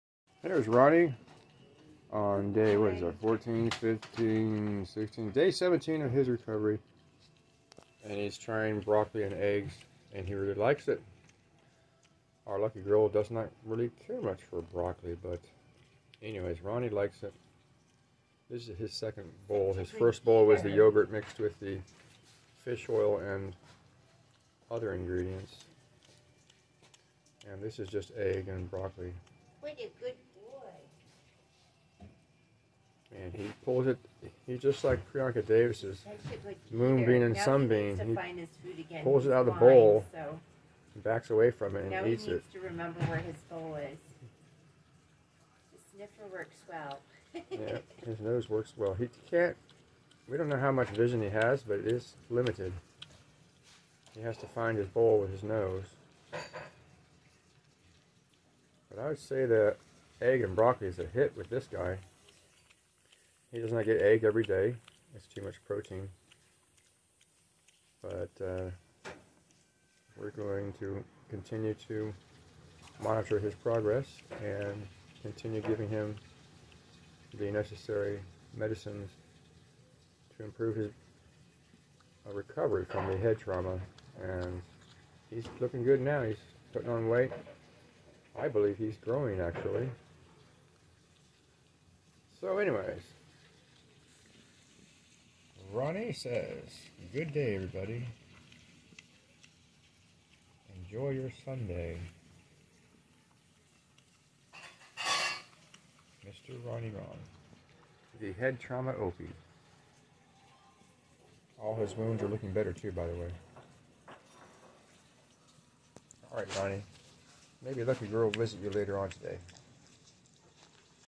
Recovering head trauma Opossum Ronny Boy on day 17. He loves his meal of egg and broccoli.
Recovering head trauma Opie Ronny Boy on day 17. He loves his meal of egg and broccoli. He has made great progress, I believe, because of the 2400 mg pill of fish oil for humans we give him twice a day.